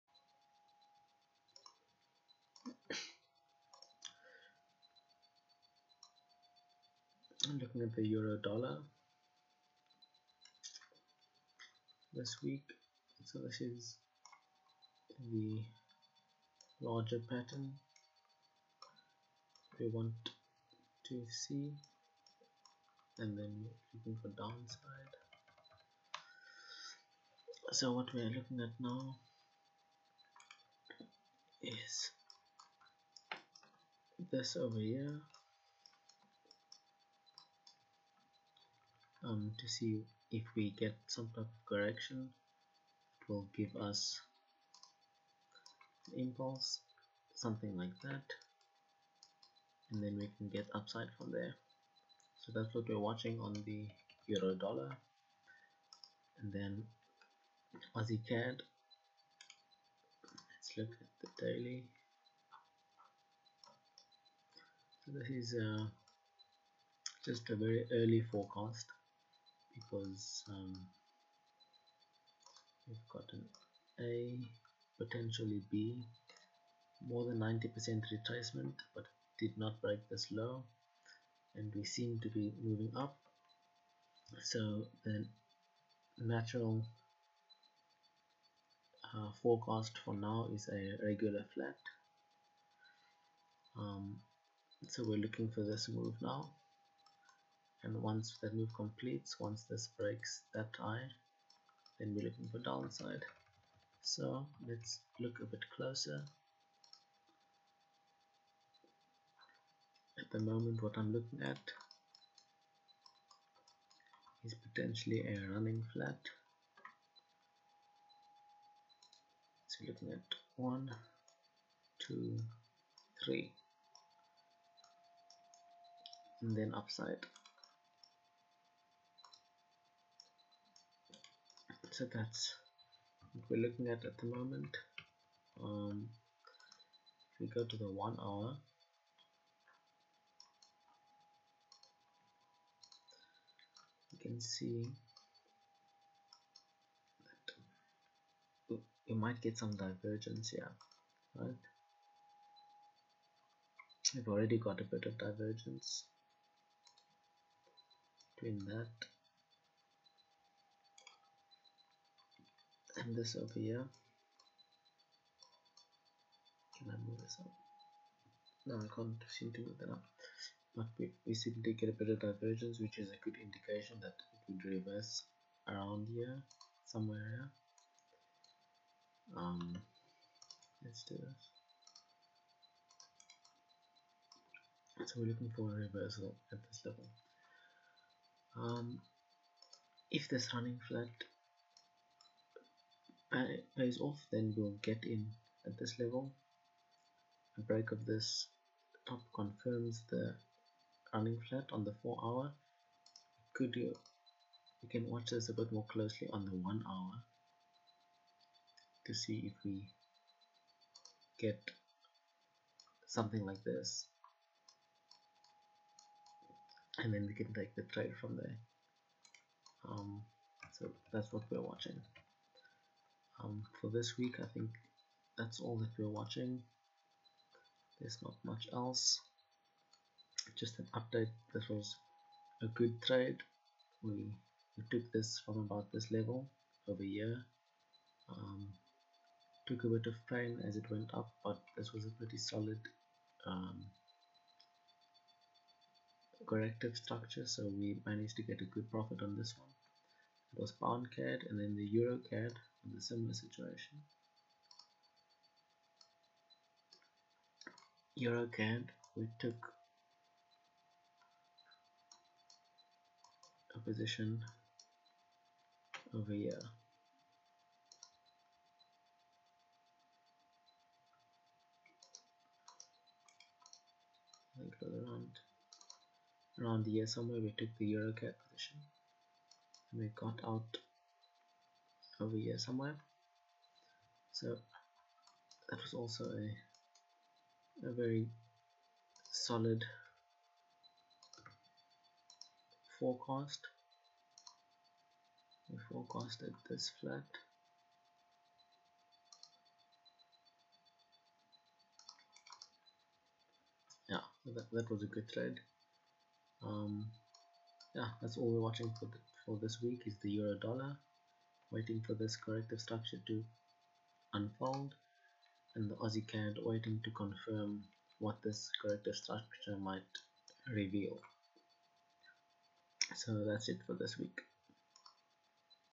Forex Analysis | June 06 - 12 | 2022
A look at trading setups I have identified for the coming week.